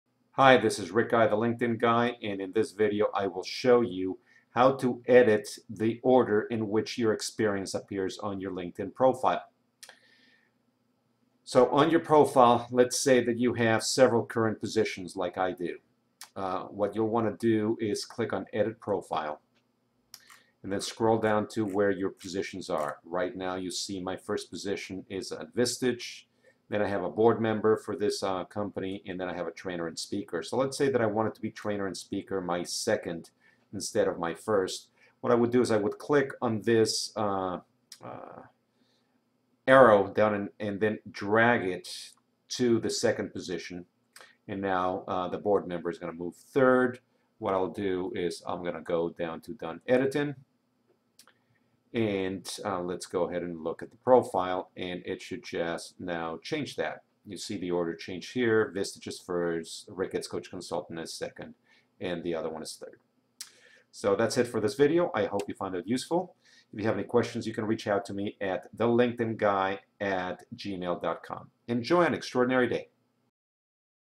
How to rearrange the order of your LinkedIn positions
In this video I show you how to re-arrange the position order of your LinkedIn previous experiences. Veronessa